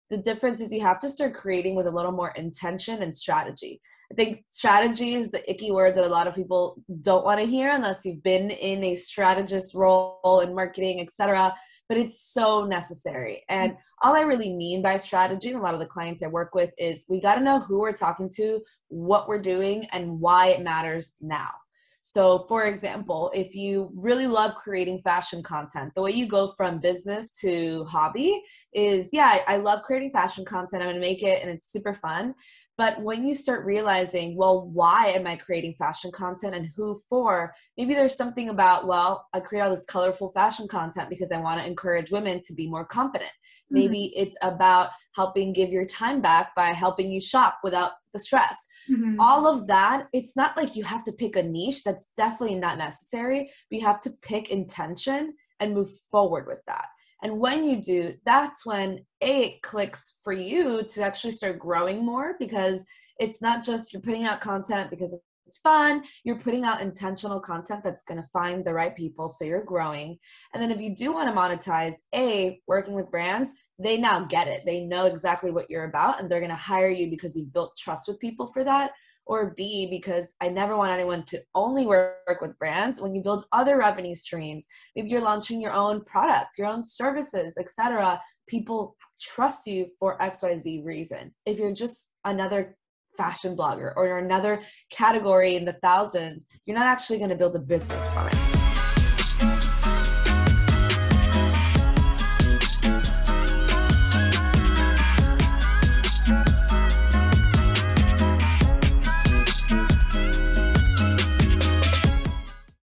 Creating Strategy As An Influencer
This week, we sit down with owner + CEO of @influencewithimpact and Award Winning coach, @lissettecalv who is giving us a deep dive into all things influencer marketing! Tune in for more on negotiating rates, managing a team and so much more!